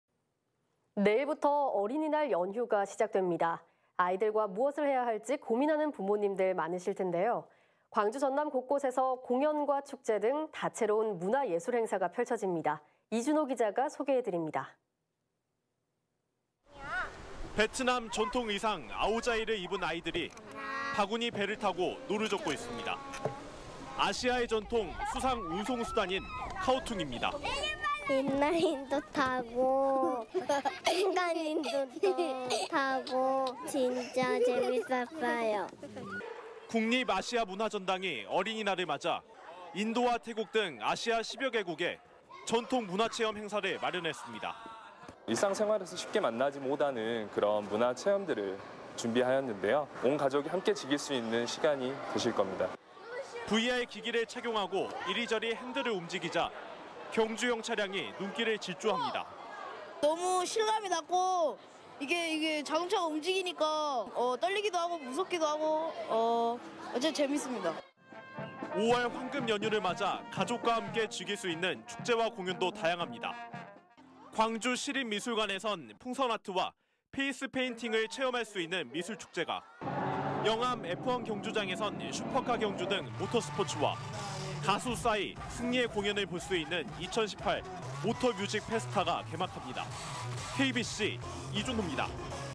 어린이날 황금연휴...'어디를 가볼까'
내일부터 어린이날 연휴가 시작됩니다. 아이들과 무엇을 해야 할지 고민하는 부모님들 많으실 텐데요.

광주*전남 곳곳에서 공연과 축제 등 다채로운 문화예술 행사가 펼쳐집니다.

이준호 기자가 소개해드립니다.